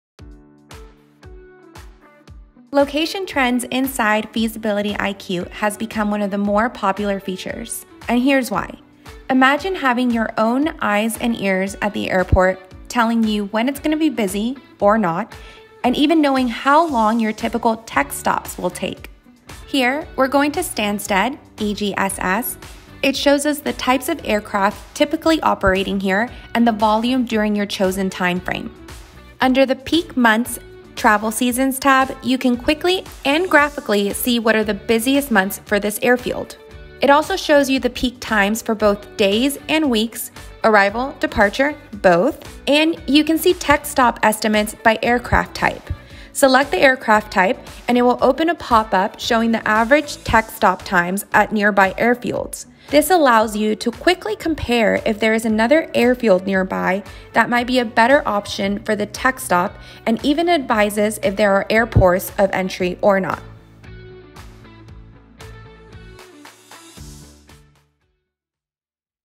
Find Location Trends at all Airports with Feasibility-IQ
Imagine having your own eyes and ears at the airport, telling you when it’s going to be busy or not, and even knowing how long your typical tech stops will take. This is what the Location Trends feature does for you within Feasibility-IQ - and it's no wonder it's become one of the more popular features.

Check it out and sign up for a free 30-day trial